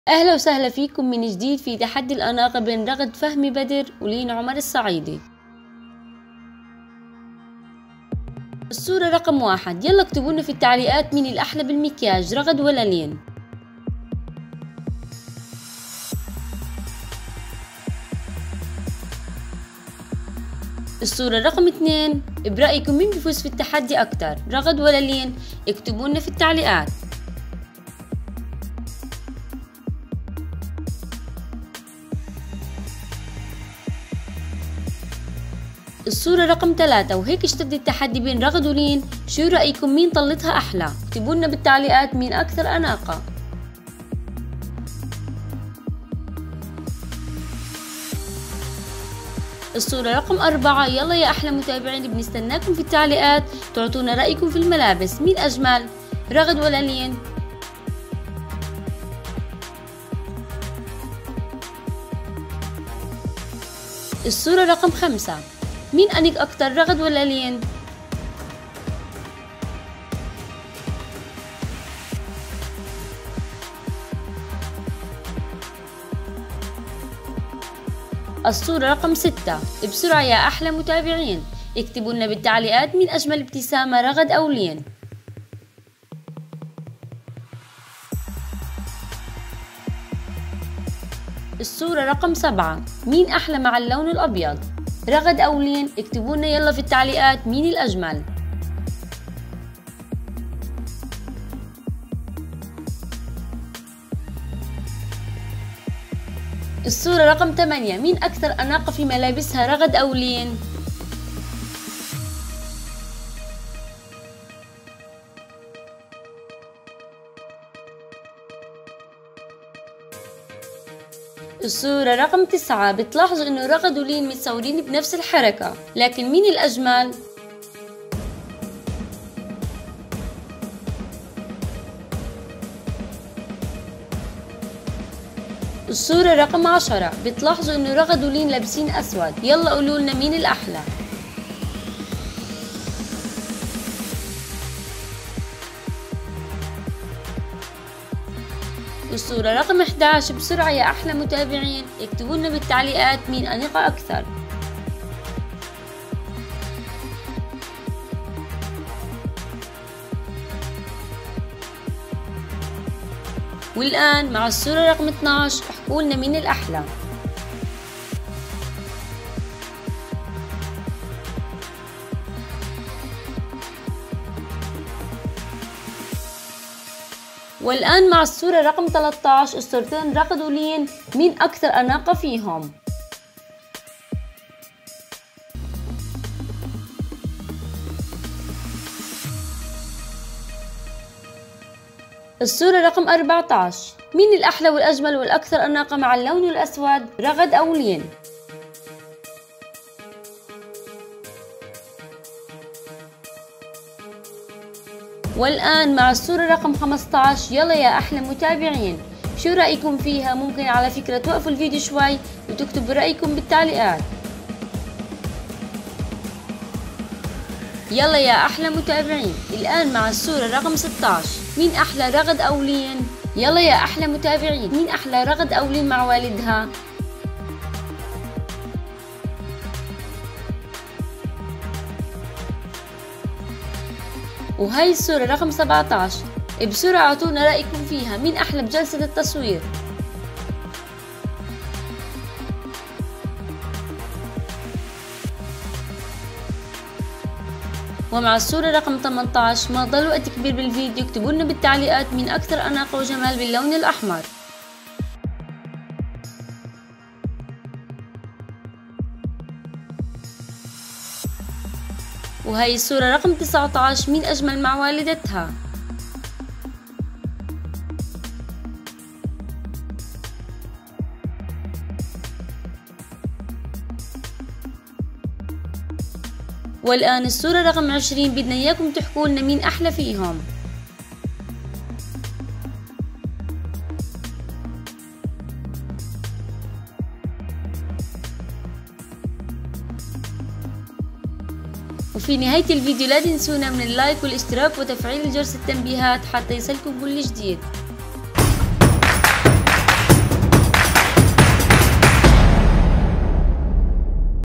رغد فهمي ولين الصعيدي في تحدي الأناقة .. مين فاز ؟
مين برأيكم بفوز بالتحدي؟ رغد اخت روان وريان نجمة قناة Ragoode رغد اخت روان وريان VS لين عمر الصعيدي أخت مايا الصعيدي ونجمة قناة Maya & Leen TV - مايا و لين الصعيدي ، تحدي الأناقة بين رغد فهمي اخت روان وريان ولين الصعيدي ابنة الفنان عمر الصعيدي منشد قناة نون || تابعوني على انستقرام